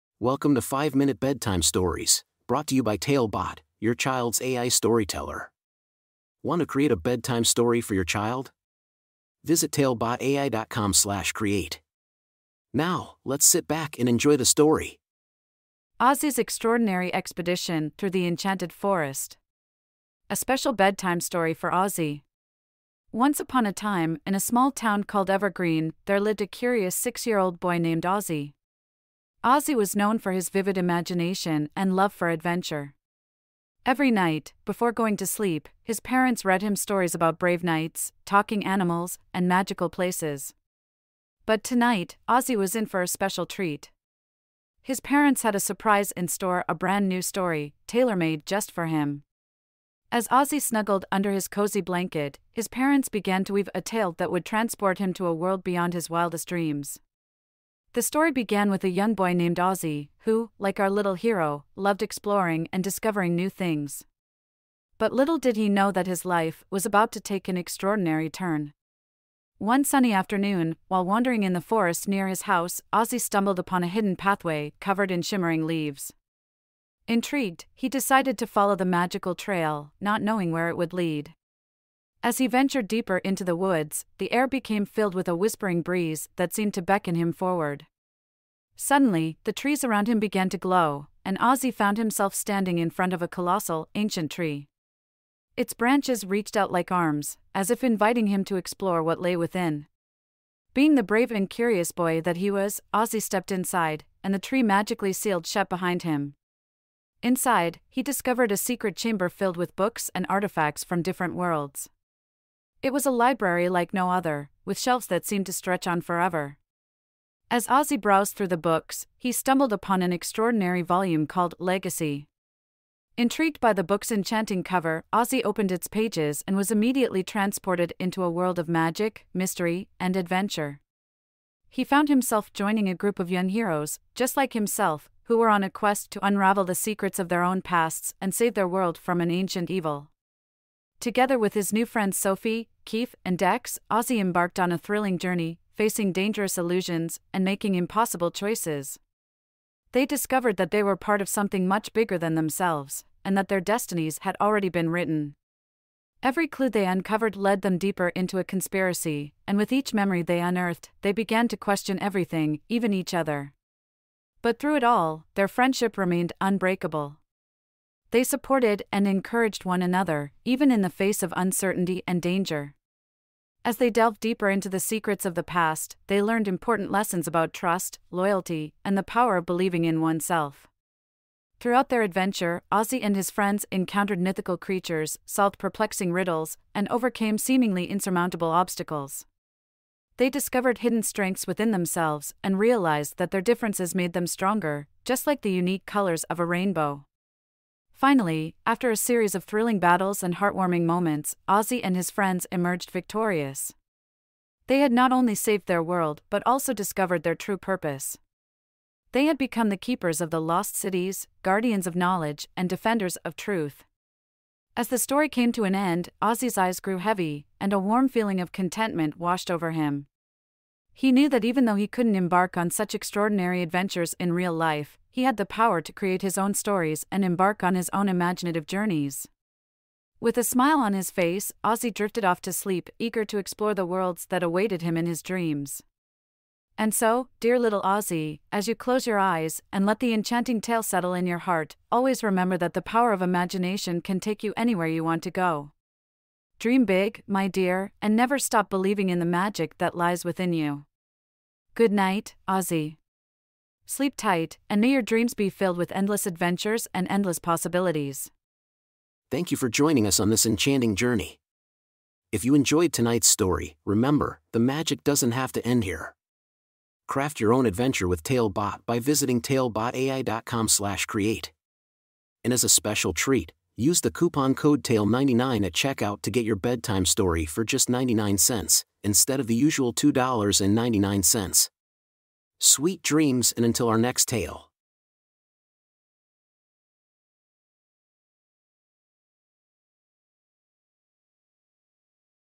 Ozzy's Extraordinary Expedition through the Enchanted Forest | 5 Minute Bedtime Stories
Embark on an extraordinary adventure through the Enchanted Forest with Ozzy. Join him as he discovers ancient secrets, faces mythical creatures, and learns the power of imagination. Get lost in this magical tale tonight!